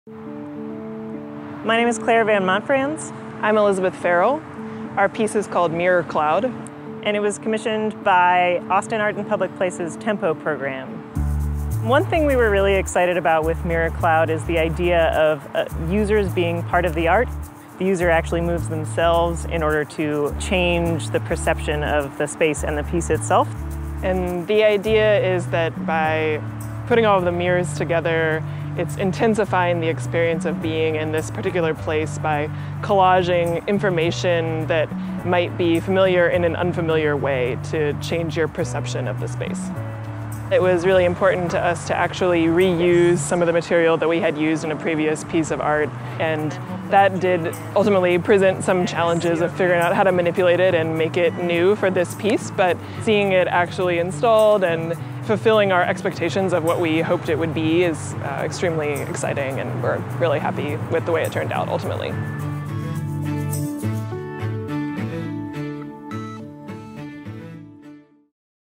Mirror Cloud | by Elizabeth Farrell & Clare van Montfrans
"Mirror Cloud" is a temporary public art installation commissioned by Austin Art in Public Places TEMPO Project.